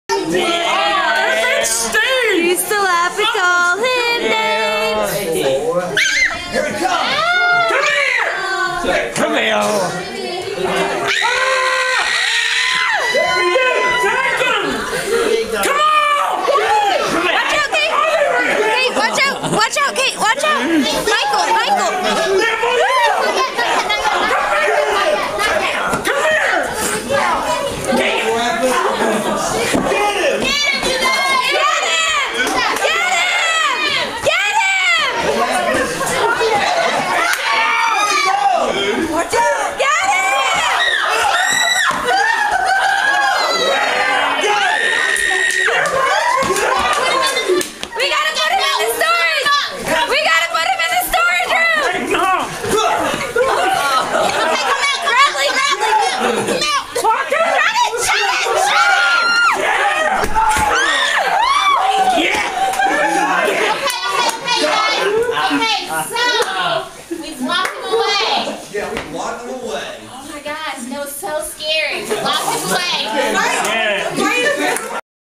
Krampus Chaos - 2013
In an attempt to capture the Christmas Demon, perhaps these friends end up with more than they bargained for...

This occurred at a day program for young adults with developmental and intellectual disabilities.  No Krampus Demons were harmed during the filming of this encounter.

April 2013
Nashville, Tennessee